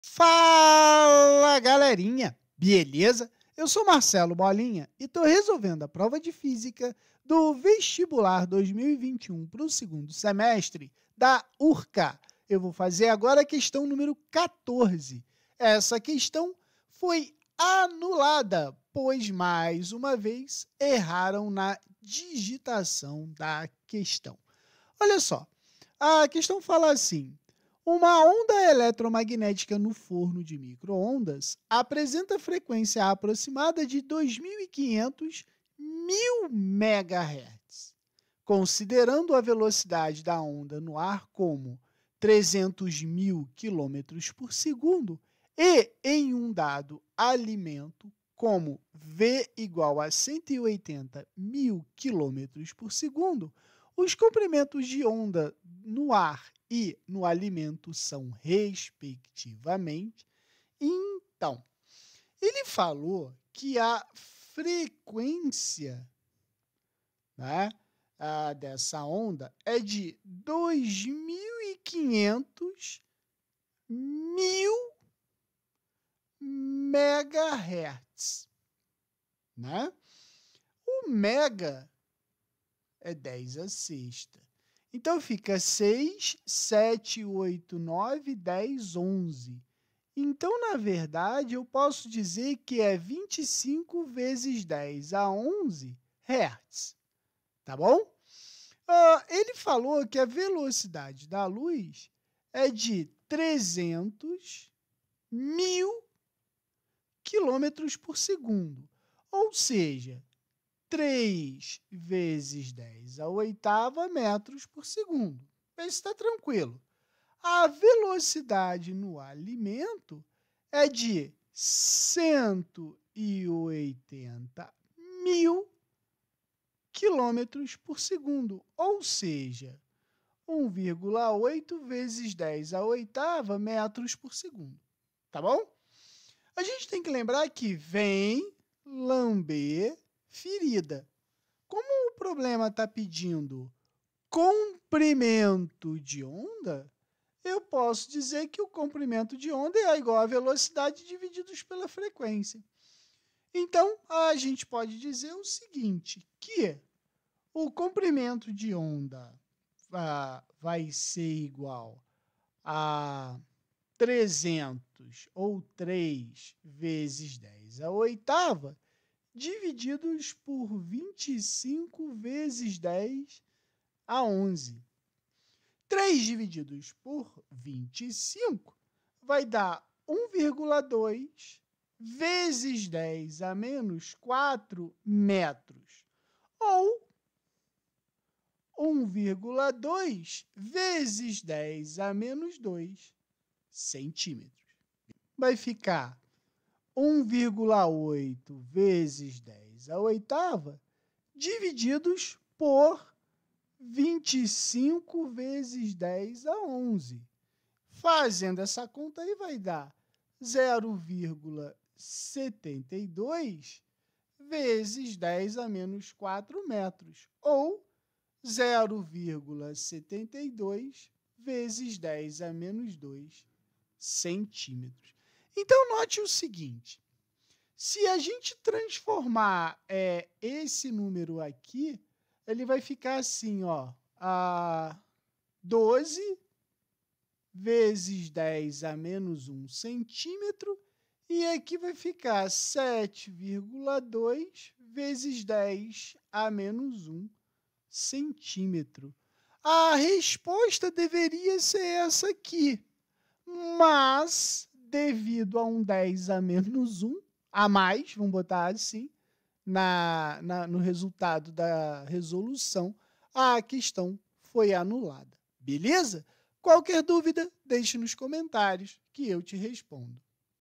Resolução da prova de FÍSICA – URCA – 2021/2 – Questão:14(ANULADA).
14-(ANULADA) Uma onda eletromagnética no forno de micro-ondas apresenta frequência aproximada de 2.500 mil mega-hertz (MHz). Considerando a velocidade da onda no ar como car = 300 mil km/s e em um dado alimento como v = 180 mil km/s, os comprimentos de onda no ar e no alimento são, respectivamente
a) 10 cm e 6 cm.
b) 12 cm e 7 cm.
c) 15 cm e 9 cm.
d) 25 cm e 20 cm.
e) 30 cm e 18 cm.